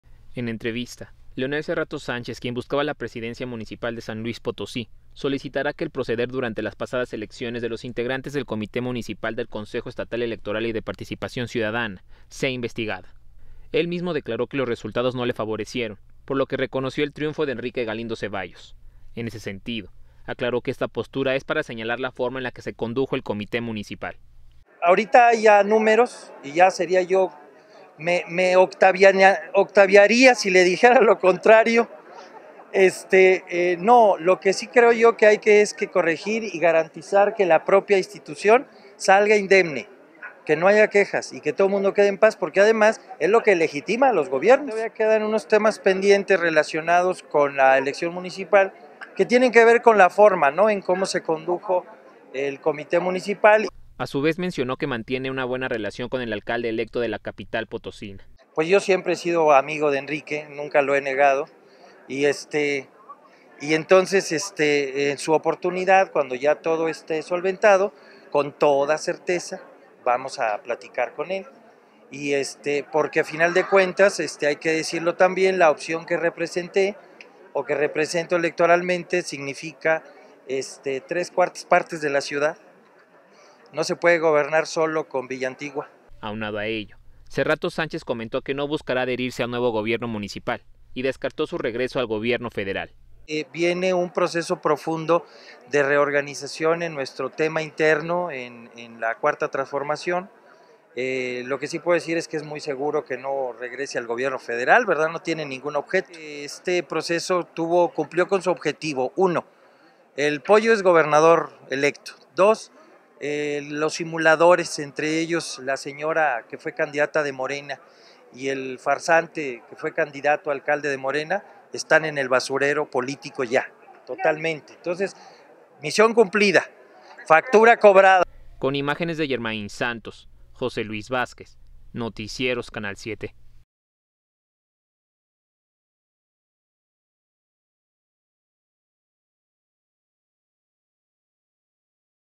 Leonel Serrato reconoce victoria de Enrique Galindo.
• Descartó regresar al Gobierno Federal.

SLP.- En entrevista, Leonel Serrato Sánchez, quien buscaba la presidencia municipal de San Luis Potosí, solicitará que el proceder durante las pasadas elecciones de los integrantes del Comité Municipal del Consejo Estatal Electoral y de Participación Ciudadana, sea investigada.

Él mismo declaró que los resultados no le favorecieron, por lo que reconoció el triunfo de Enrique Galindo Ceballos; en ese sentido aclaró que esta postura es para señalar la forma en la que se condujo el Comité Municipal.

“Ahorita ya hay números, y me Octaviaría si le dijera lo contrario, no, lo que sí creo yo es que hay que corregir y garantizar que la propia institución salga indemne, y que no haya quejas, que todo mundo quede en paz, porque es lo que legítima los gobiernos. (…) Quedan unos temas pendientes relacionados con la elección municipal y qué tiene que ver con la forma, en cómo se condujo el Comité Municipal”, agregó.

A su vez, mencionó que mantiene una buena relación con el alcalde electo de la capital potosina.

“Yo siempre he sido amigo de Enrique, nunca lo he negado, y entonces en su oportunidad, cuando ya todo esté solventado, con toda certeza, vamos a platicar con él, porque a final de cuentas hay que decirlo, también la opción que represente o que represento electoralmente, significa 3/4 partes de la ciudad, y no se puede gobernar solo con Villa Antigua”, dijo.

Aunado a ello, Serrato Sánchez comentó que no buscará adherirse al nuevo gobierno municipal y descartó su regreso al Gobierno Federal.

"Viene un proceso profundo de reorganización en nuestro tema interno en la cuarta transformación, lo que sí puedo decir es que es muy seguro que no regrese al Gobierno Federal, no tiene ningún objeto... este proceso cumplió con su objetivo, uno: el pollo es gobernador electo; dos: los simuladores, entre ellos la señora que fue candidata de morena y el farsante que fue candidato a alcalde de Morena están en el basurero político, misión cumplida, factura cobrada", culminó.